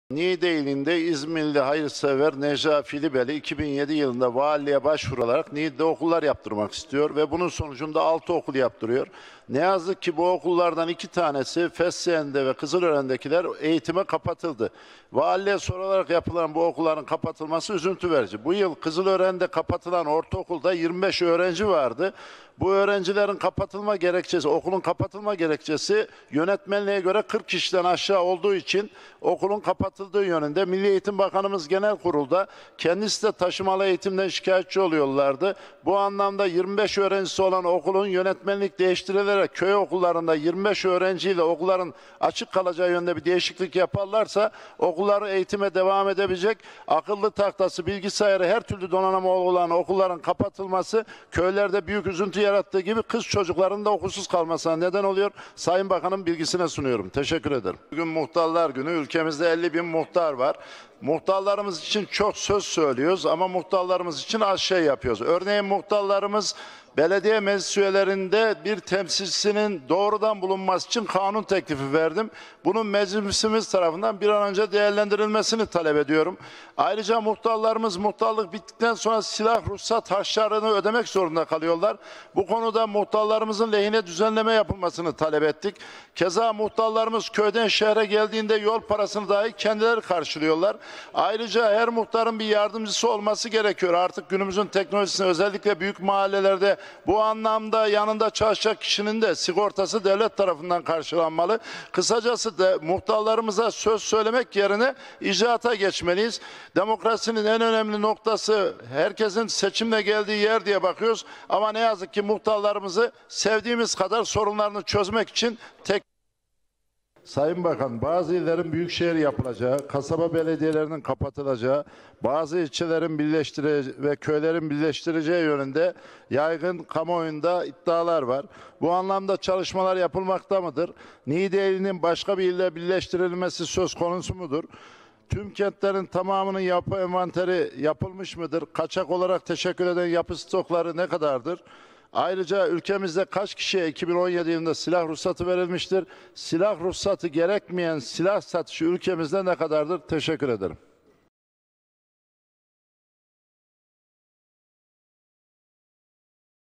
Geçen Hafta Mecliste Neler Konuşuldu?
Milletvekillerimiz yurttaşlarımızın meclisteki sesi olmaya devam ediyor. Niğde Milletvekilimiz Ömer Fethi Gürer'in bu haftaki meclis konuşmaları.